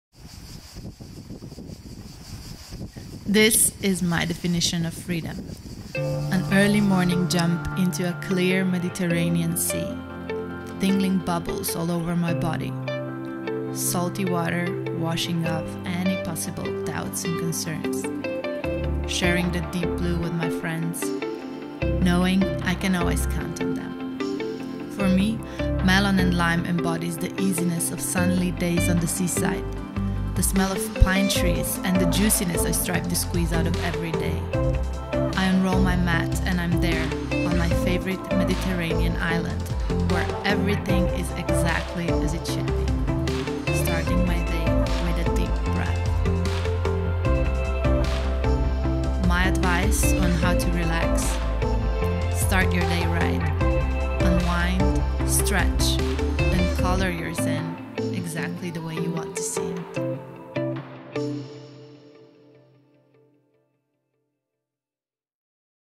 Melon & Lime Color Your Zen
Melon & Lime's CEO Rebeka describes her perfect day and shares a tip on how to feel good every day.